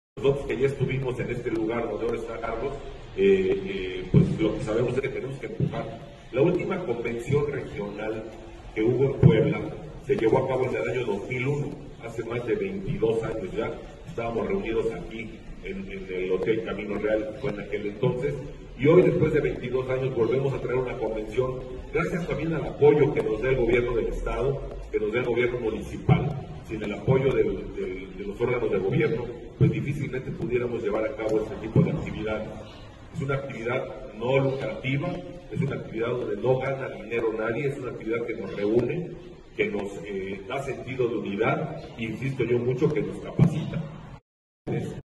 El Colegio de Contadores públicos de Puebla llevará a cabo la convención Regional Centro Istmo
Del 17 al 19 de mayo de 2023 en el Hotel Presidente Intercontinental, el Colegio de Contadores públicos del Estado de Puebla llevará a cabo la convención regional Centro Istmo Peninsular.